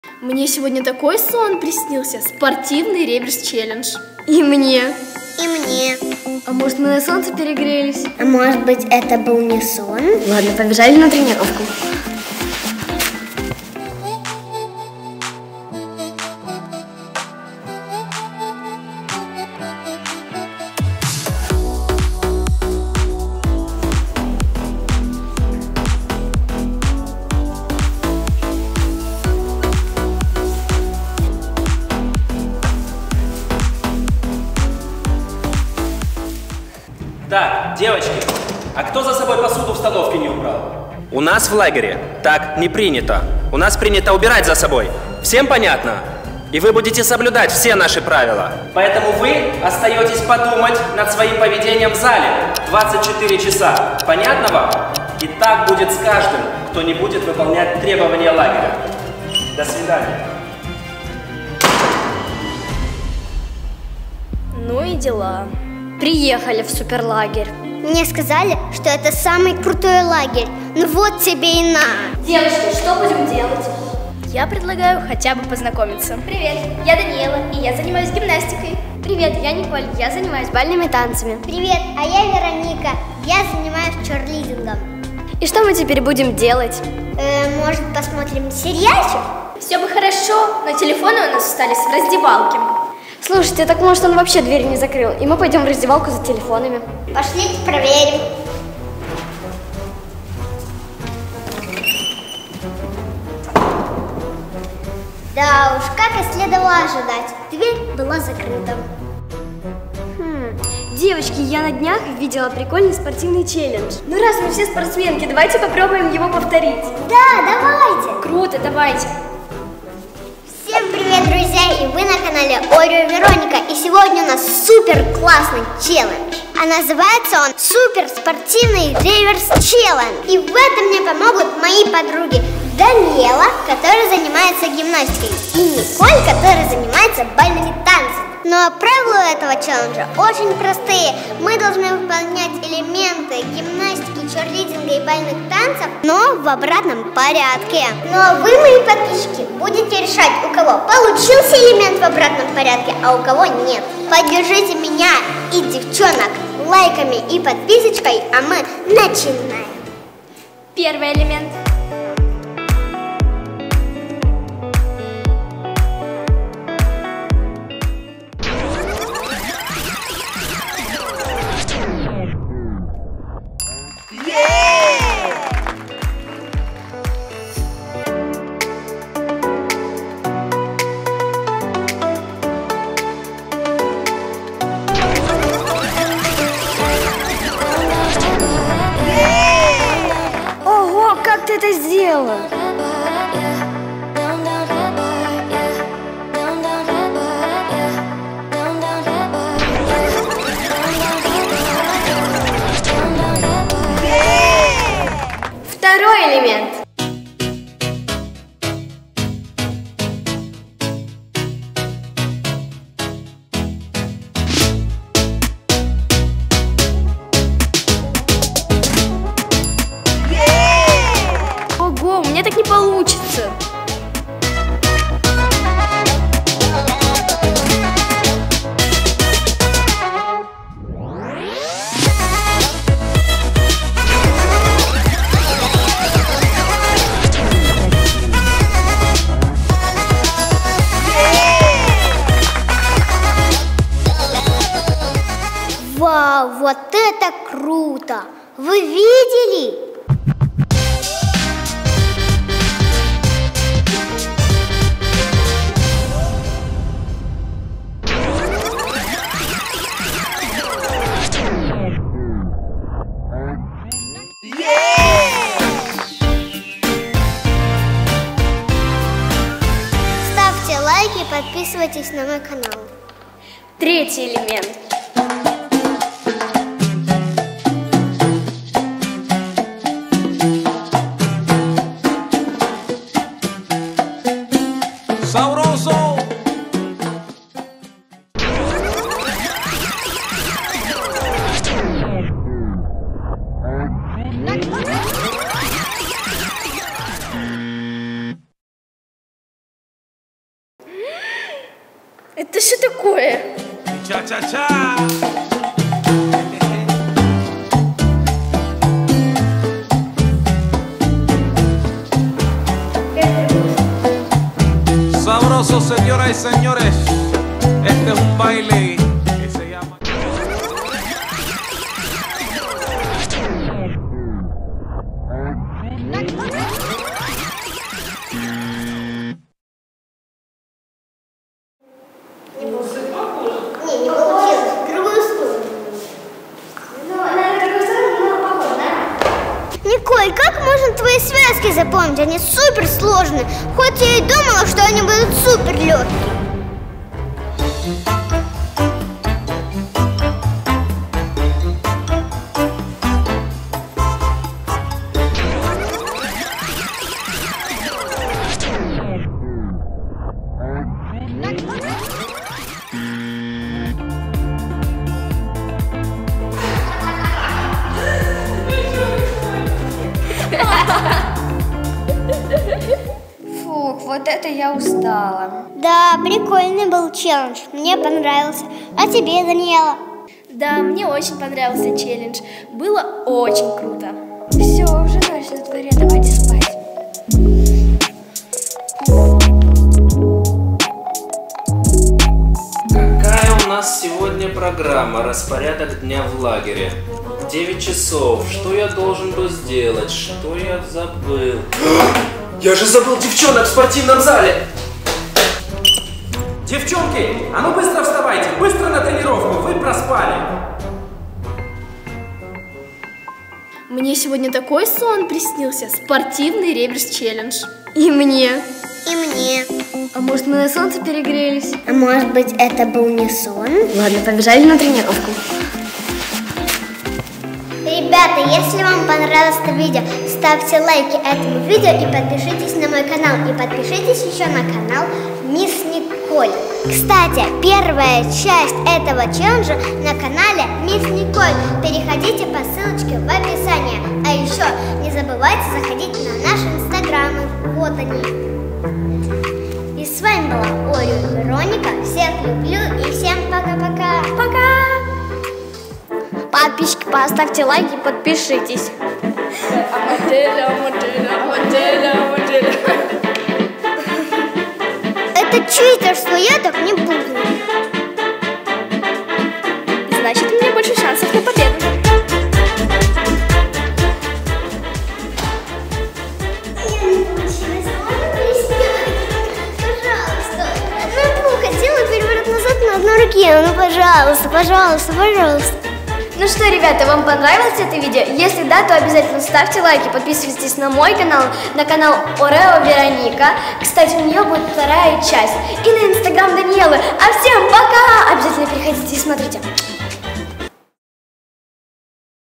24 Часа Реверс челлендж | Мисс Николь и Oreo Veronika
Ура! Ведь лето и много свободного времени! Я наконец-то поехала в спортивный лагерь! Я познакомилась там с классными ребятами и девчонками! Но больше всех я подружилась с Даниэллой и Мисс Николь! Да! Та самая Николь гимнастка! Мы очень круто провели время вместе! Нам было очень весело. А чем мы там занимались? КОнечно же снимали для вас видосик. 24 Часа Реверс челлендж | Мисс Николь и Oreo Veronika.

АКРОБАТИЧЕСКИЙ РЕВЕРС ЧЕЛЛЕНДЖ в СПОРТИВНОМ ЛАГЕРЕ // Гимнастка VS Черлидерша VS Танцовщица

Желаю Вам классного просмотра!

🎧 По этой ссылке 30 дней бесплатного доступа к огромной библиотеке классной музыки и звуков к вашим видео.